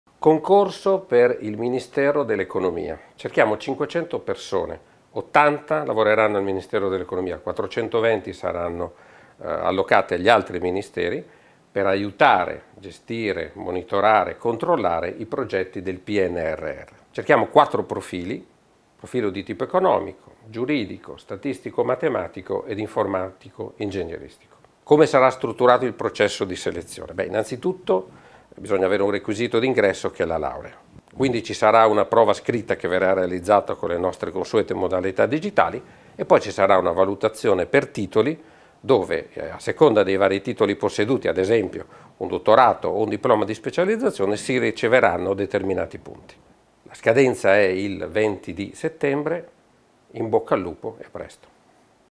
CONCORSO MEF PER PNRR
Bando di concorso pubblico, per il reclutamento di 500 unità di personale non dirigenziale a tempo determinato 80 da assegnare al Ministero dell'Economia e delle Finanze e 420 alle PA Centrali titolari di interventi previsti nel Piano Nazionale di Ripresa e Resilienza.

A cura di Alessandra Flick
Riprese Riccardo De Stefano